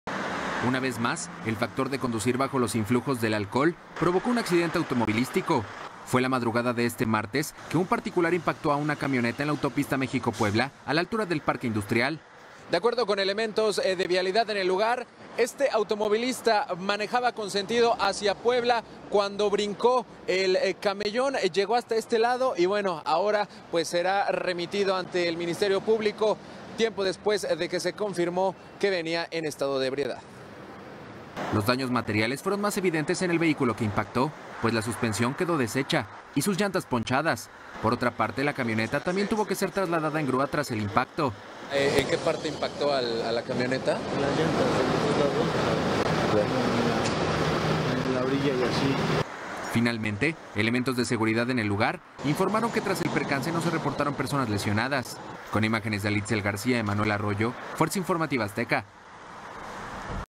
Accidente automovilístico
Un automóvil particular se impactó contra una camioneta sobre la Autopista México-Puebla a la altura del ParqueIndustrial; elementos de seguridad aseguran que el conductor venía en estado de ebriedad. 🚨